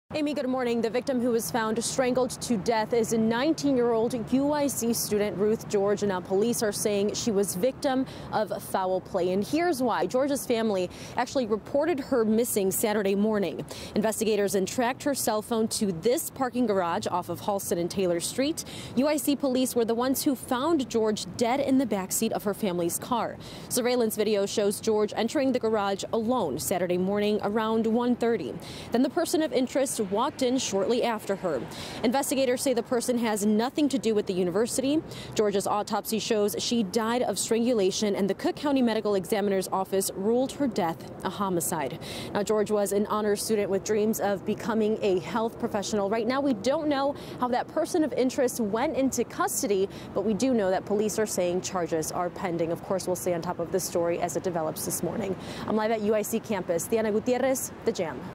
UIC student found dead in campus parking garage
A person is in police custody after a missing college student is found dead inside a University of Illinois at Chicago campus parking lot.
The investigation is ongoing.